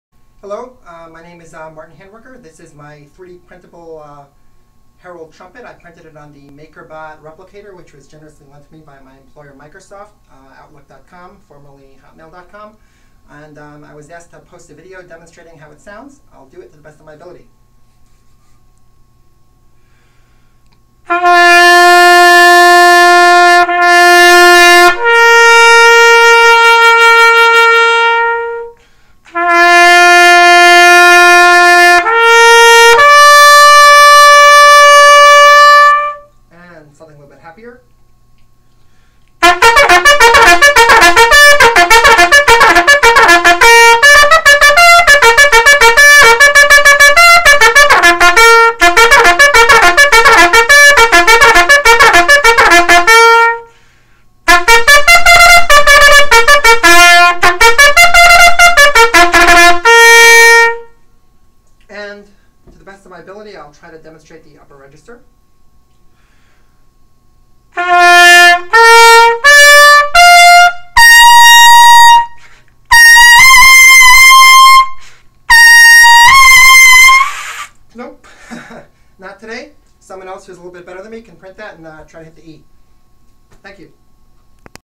3D Printed Herald Trumpet Sound Demo
This is a playable Bb Herald Trumpet. I printed it on a MakerBot as 10 pieces which were assembled and then bonded together using acetone. It plays about 20 cents flat, likely because my Bach 3C mouthpiece doesn't insert as far as my design intended.